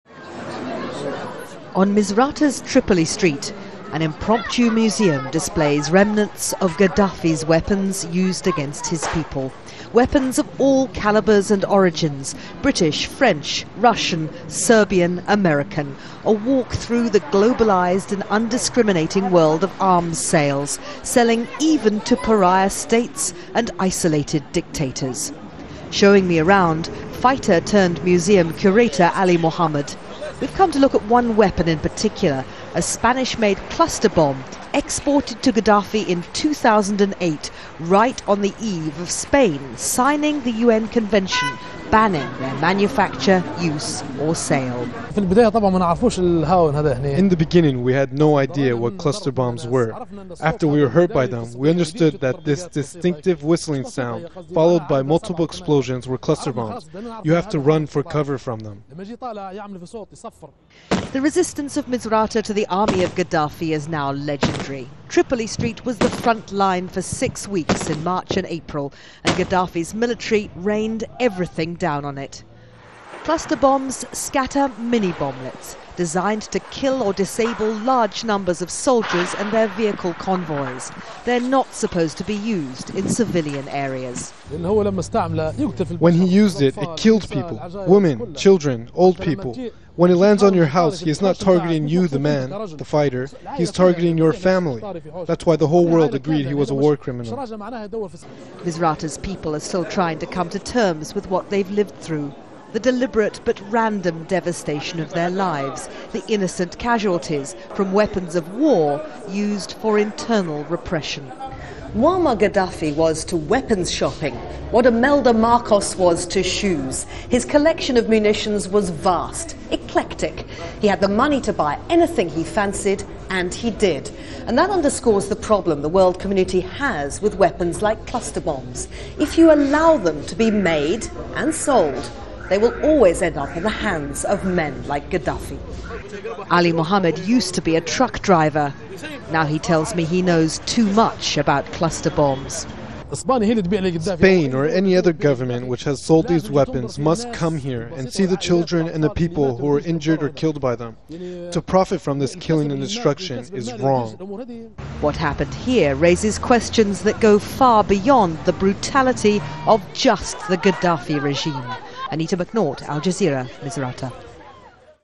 Libya displays evidence of cluster bombs
In Misrata, a coastal city that saw some of the fiercest fighting during the uprising against Muammar Gaddafi this spring, residents can now peruse evidence of the weapons used against them.

Among the shells and canisters from Britain, France, Russia, Serbia and others are Spanish-made MAT-120 "mortar cargo bombs," a cluster munition that became infamous when journalists and Human Rights Watch discovered its use by Gaddafi's forces during Misrata's siege.

Not long after Spain sold the MAT-120s to Libya in 2008, it signed the international ban on such weapons. Ali Muhamad, a fighter turned curator at the streetside weapons expo, said he had never heard of cluster bombs before Misrata but soon became used to their distince whistle and explosion.

"To profit from this killing and destruction is wrong," Mohamad said.

Al Jazeera's Anita McNaught reports from Misrata.